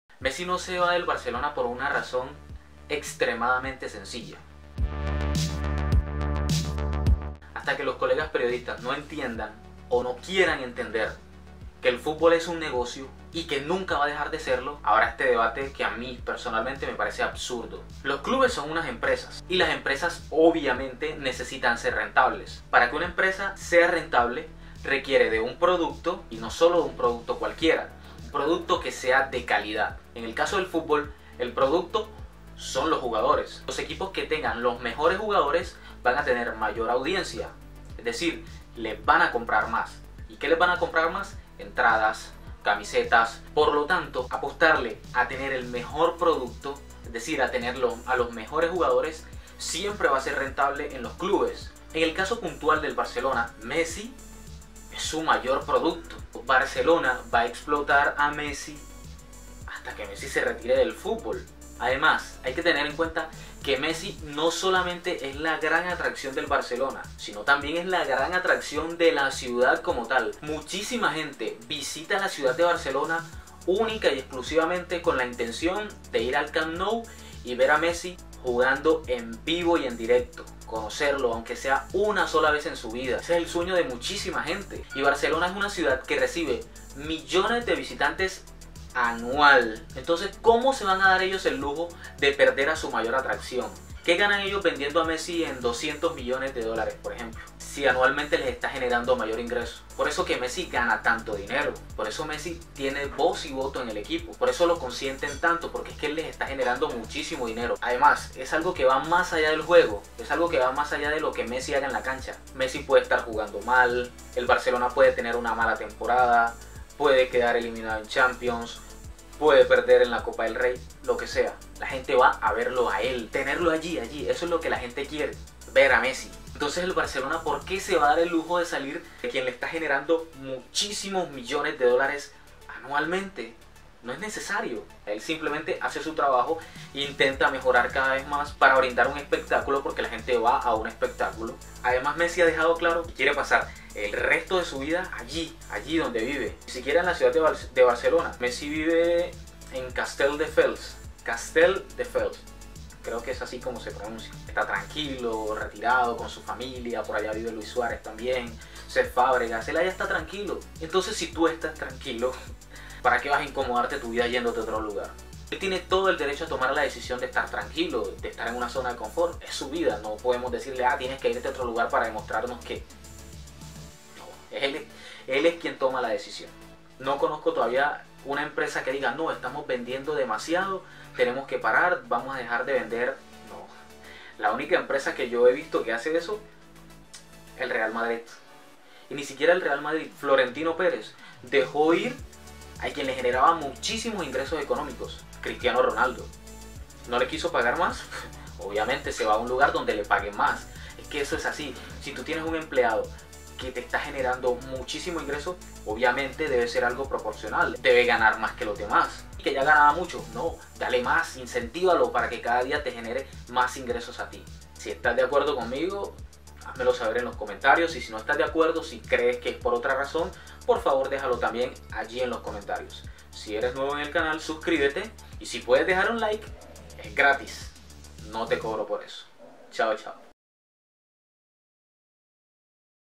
¿POR QUÉ MESSI NO SE VA DEL BARCELONA?
Existen miles de opiniones sobre este tema, pero la realidad es otra. Aquí te lo explico todo.